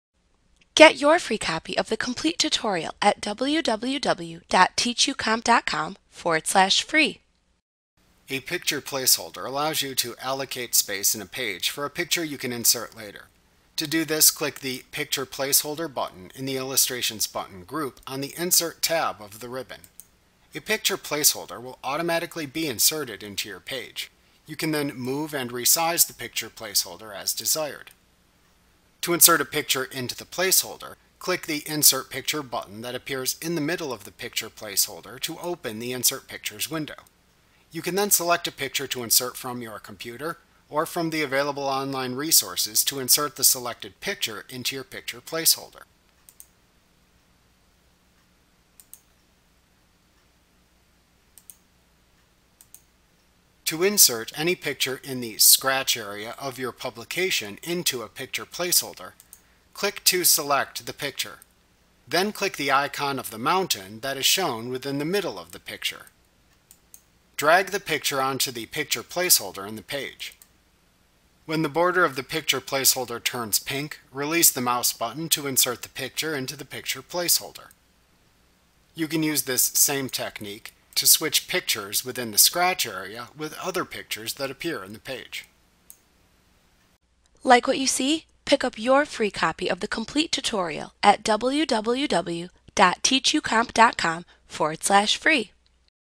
Publisher 2019 & 365 Tutorial Inserting Picture Placeholders Microsoft Training
FREE Course! Learn how to insert picture placeholders in Microsoft Publisher - the most comprehensive Publisher tutorial available. Visit us today!